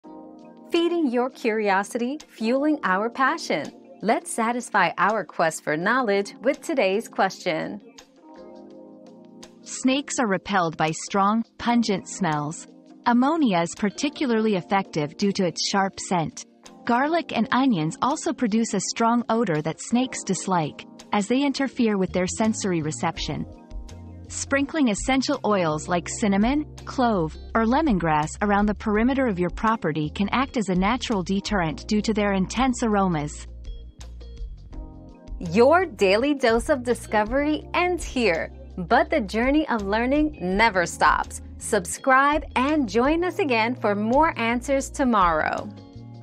What smell do snakes hate?
Repelling Snakes: Discover the Smells They Hate 👉 Snakes Hate Smells 👉 Learn about the strong and pungent smells that snakes absolutely hate. Find out how ammonia, garlic, onions, and essential oils like cinnamon, clove, and lemongrass can effectively repel snakes from your property.

Laura S. Harris (2024, January 28.) What smell do snakes hate?
    🌐 AskAbout.video